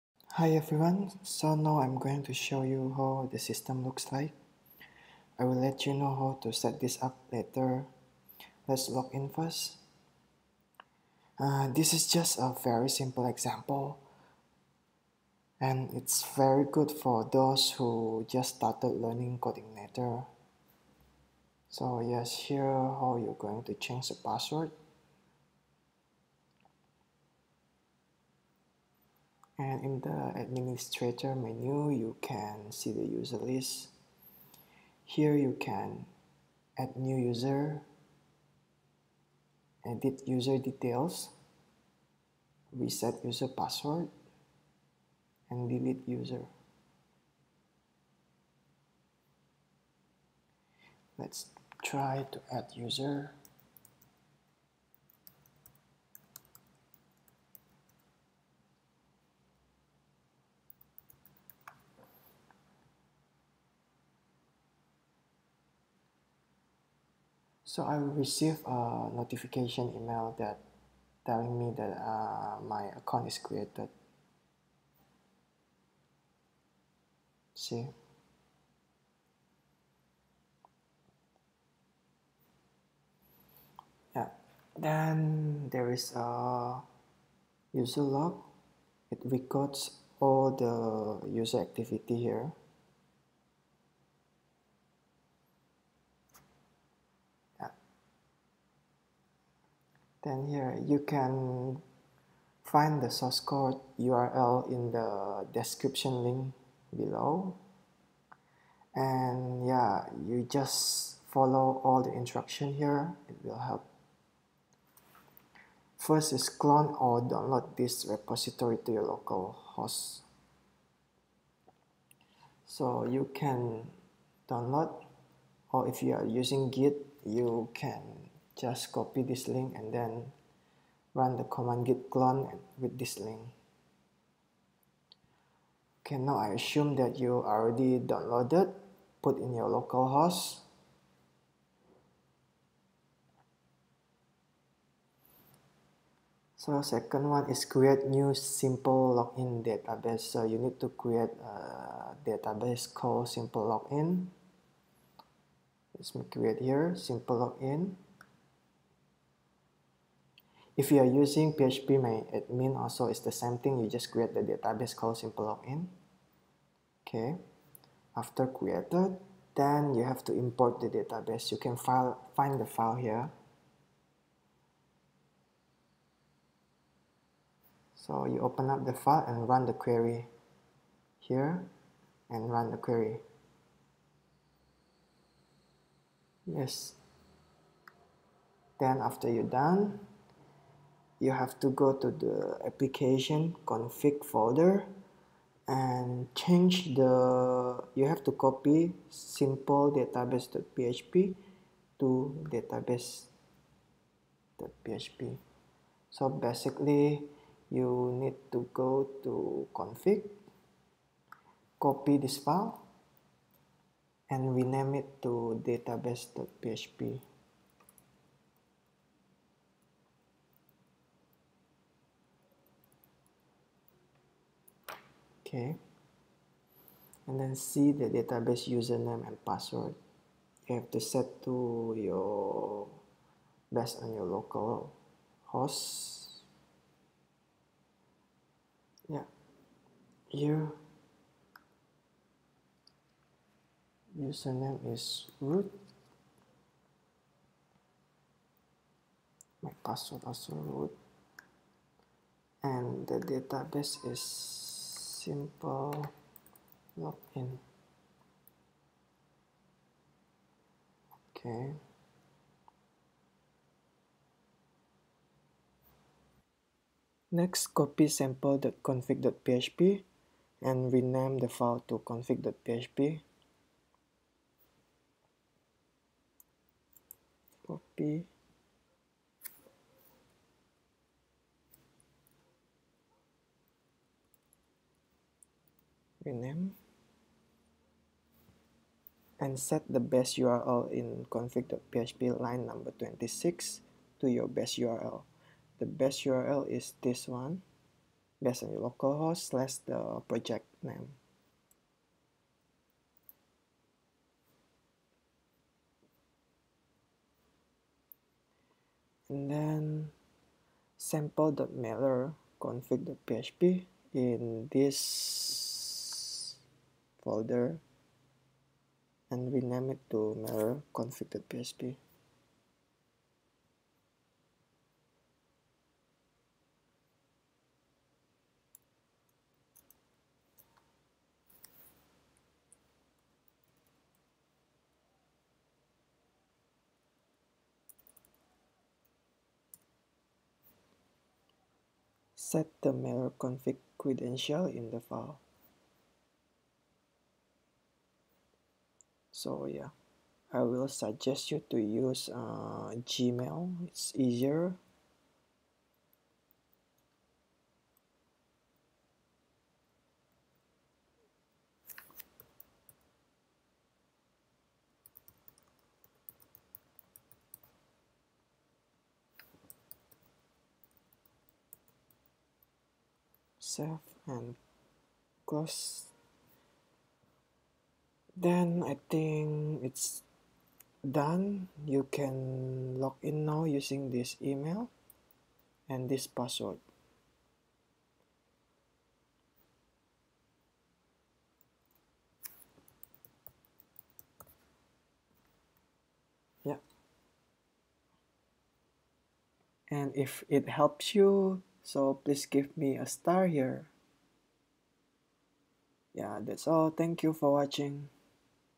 Simple User Login, User CRUD, Email Notification with Codeigniter 3
This web application is an example for those who wants to learn how to do user login and user management system using codeigniter 3.

Functions: user login, add user, edit user, delete user, reset password, change password, email notification, log activity.